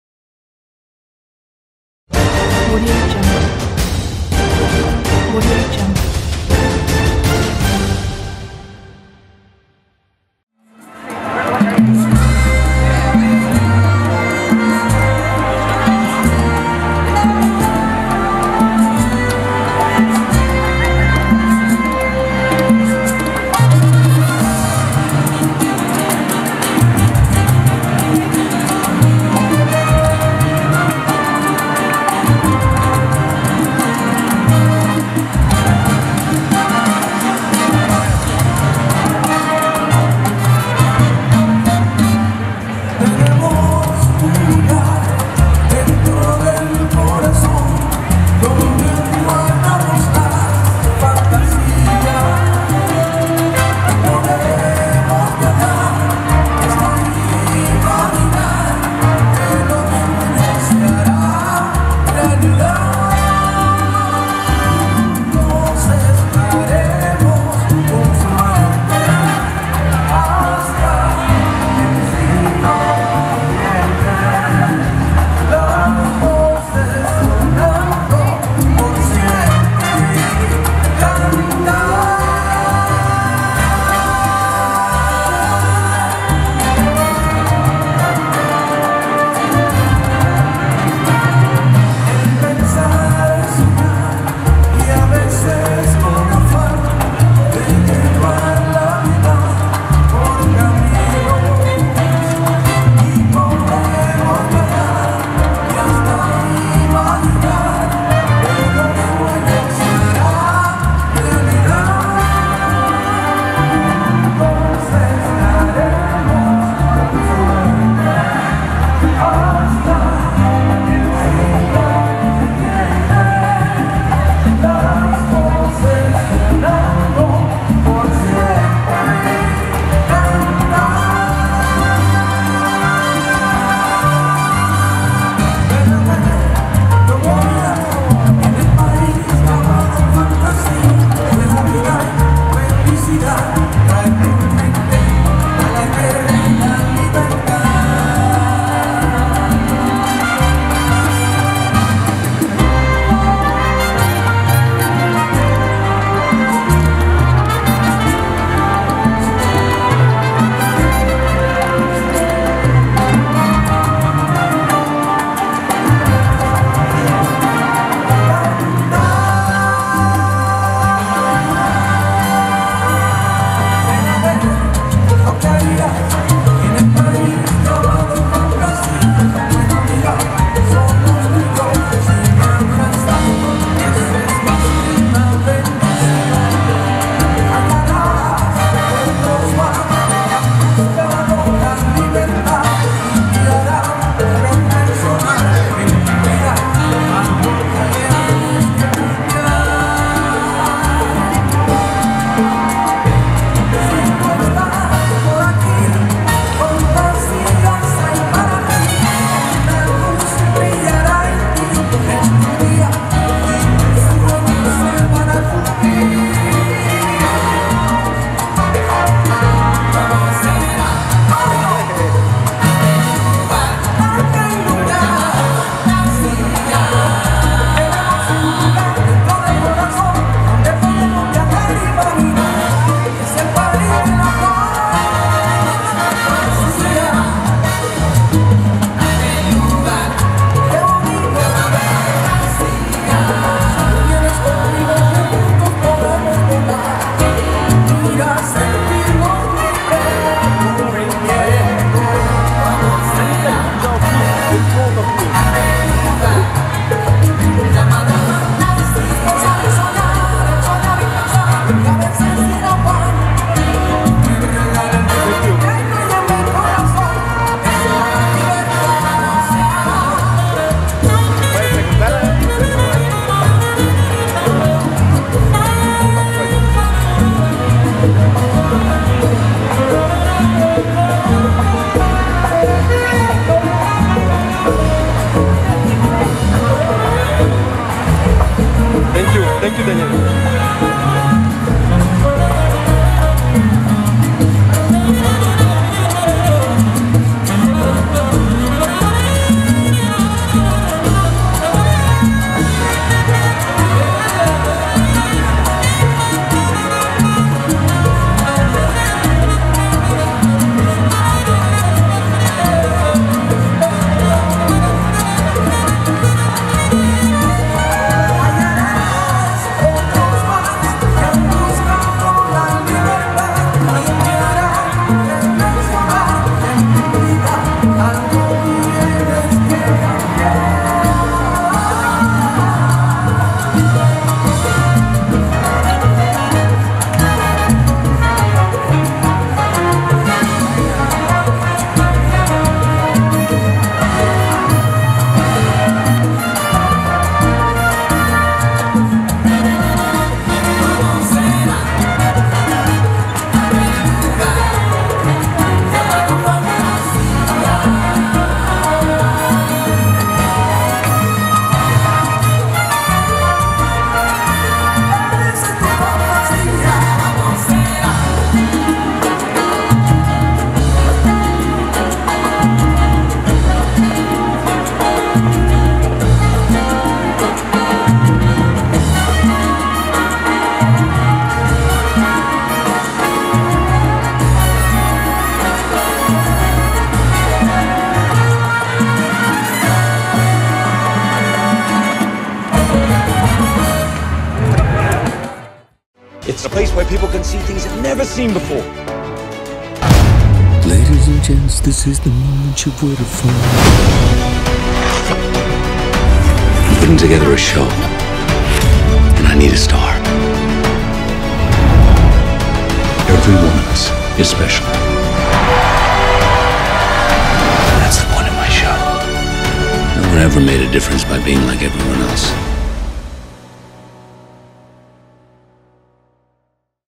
Julien & Charlotte - social dancing @ BERLIN SALSA CONGRESS 2018
It was a big honor and pleasure to be part of this amazing festival, meeting all this great international people and Salseros in the weekend of 05-08 Oktober 2018!
Sunday Night Party.
"Berlin Salsa Congress ROCK"!
Thank you Franco S.
Tickets for 2019 are already on sale!